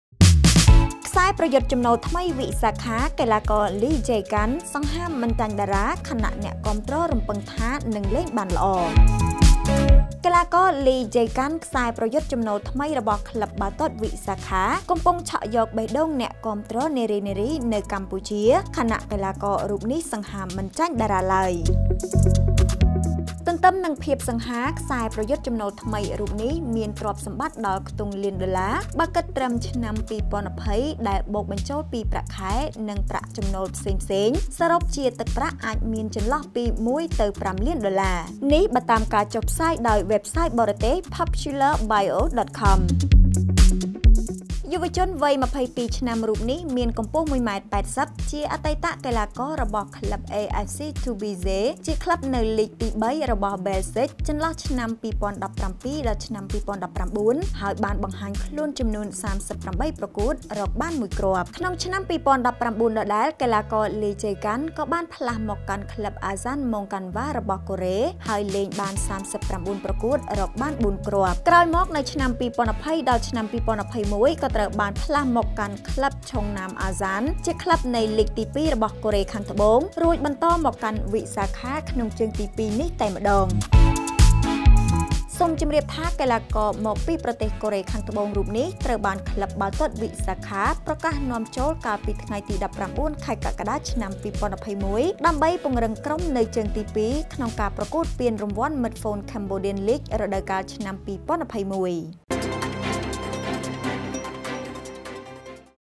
ខ្សែប្រយុទ្ធចំណូលថ្មីវិសាខា កីឡាករ Lee Jae Gun កំពុងឆក់យកបេះដូងអ្នកគាំទ្រនារីៗនៅកម្ពុជា
លោកអ្នកអាចទាក់ទងផ្ដល់ព័ត៌មានទាន់ហេតុការណ៍ 24/7 មកកាន់អង្គភាពសារព័ត៌មាន DE TIME NEWS បានតាមរយៈទូរស័ព្ទលេខ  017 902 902 (Hotline)